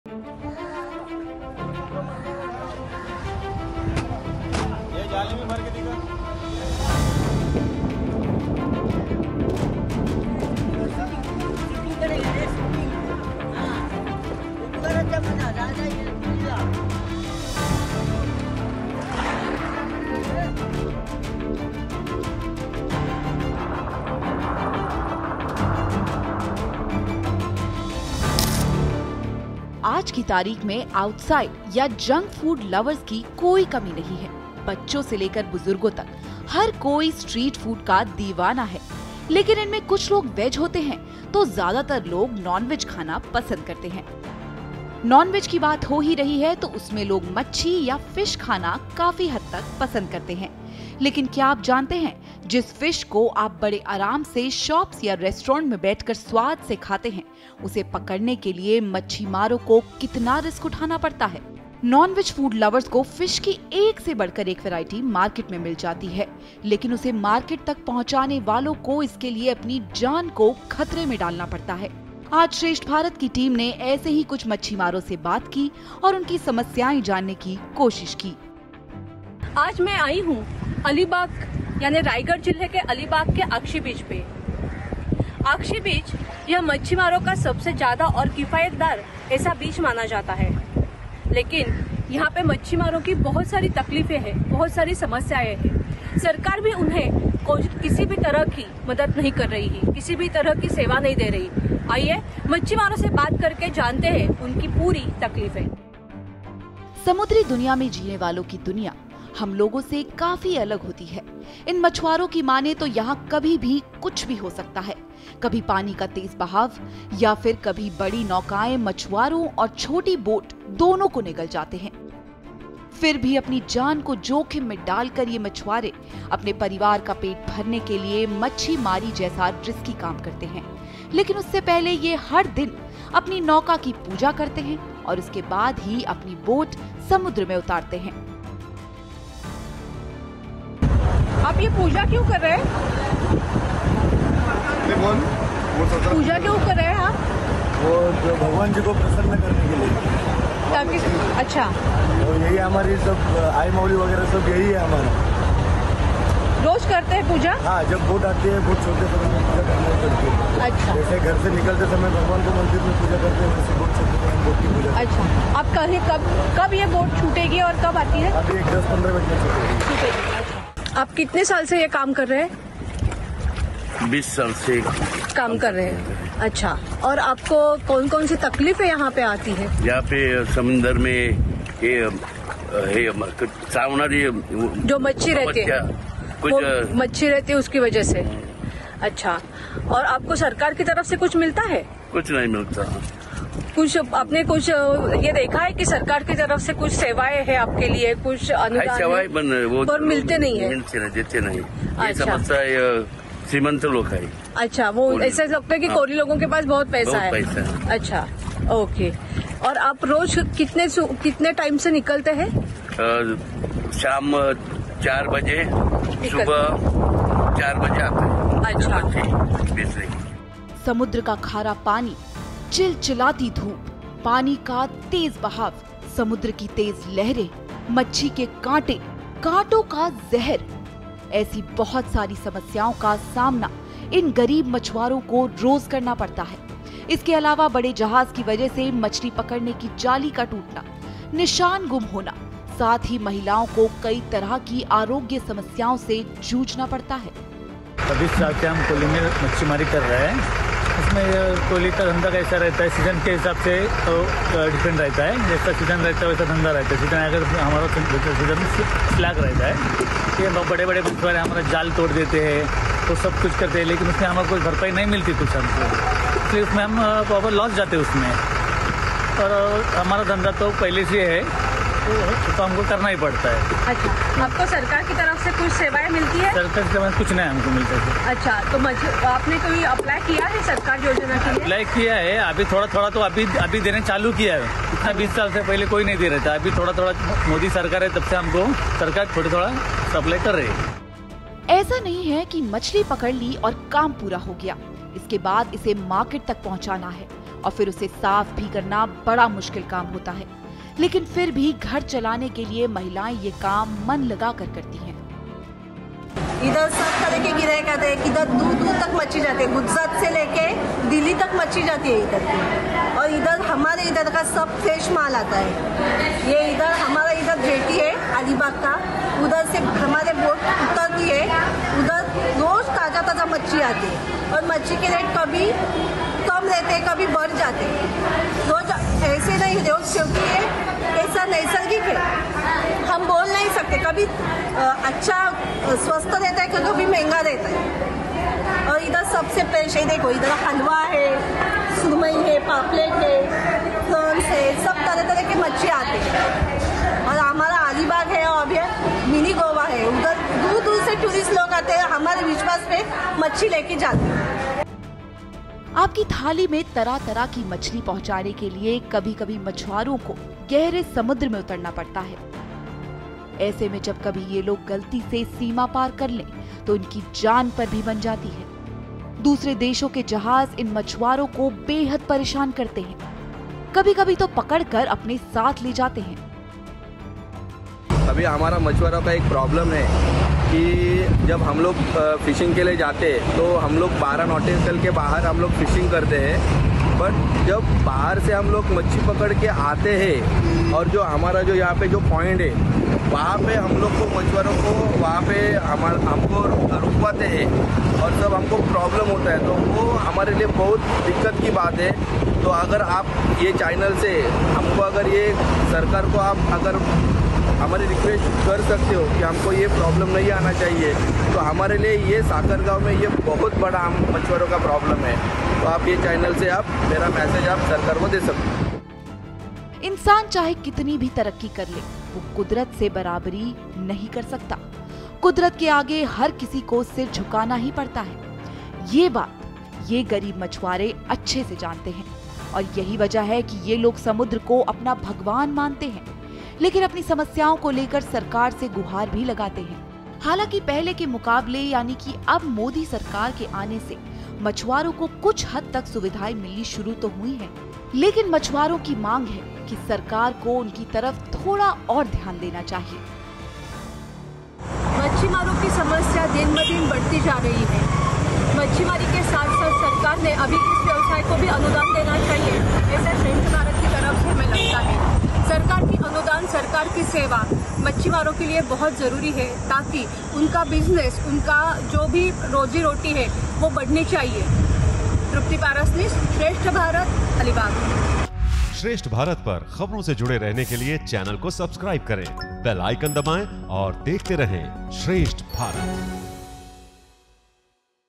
आपकी Plate तक हर Variety की मछली पहुंचाने में कितनी होती है परेशानी, सुनिए खुद मछुआरों की ज़ुबानी
समुद्री दुनिया में जीने वालों दी दुनिया हम लोगों से काफी अलग होती है. इन मछुआरों की मानें तो यहां कभी भी, कुछ भी हो सकता है. कभी पानी का तेज़ बहाव या फिर कभी बड़ी नौकाएं मछुआरे और छोटी बोट, दोनों को निगल जाते हैं. फिर भी अपनी जान को जोखिम में डाल कर ये मछुआरे अपने परिवार का पेट भरने के लिए मच्छीमारी जैसा रिस्की काम करते है. लेकिन उससे पहले ये हर दिन अपनी नौका की पूजा करते हैं और उसके बाद ही अफनी बोट समुद्र में उतारते हैं.